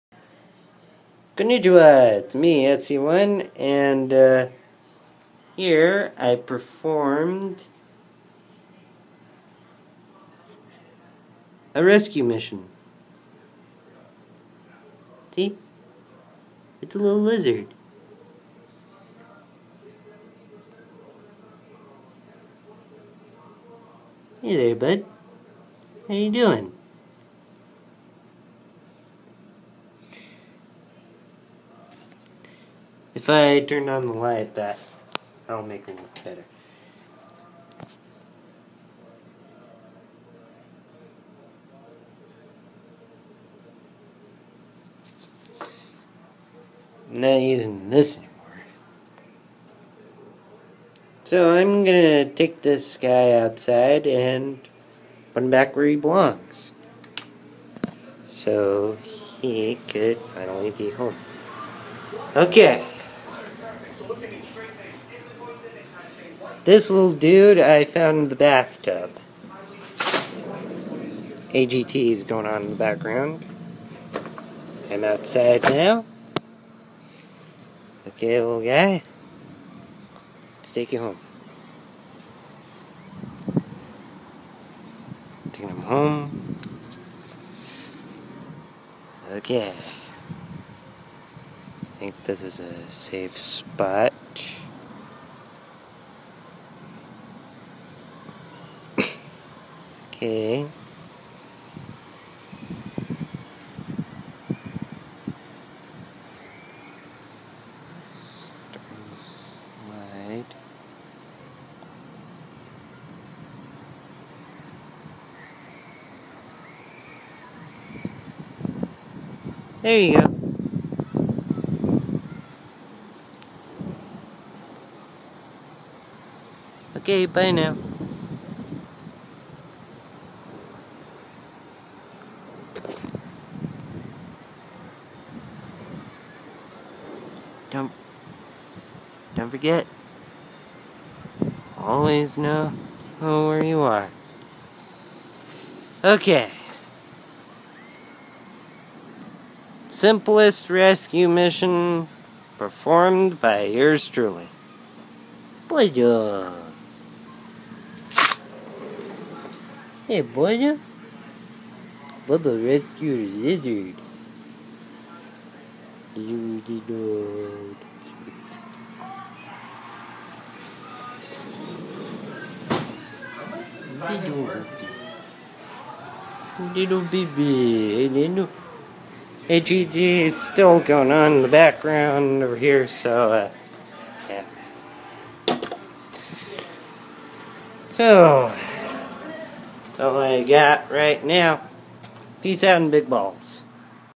A little lizard goes home
I just released a little lizard into the wild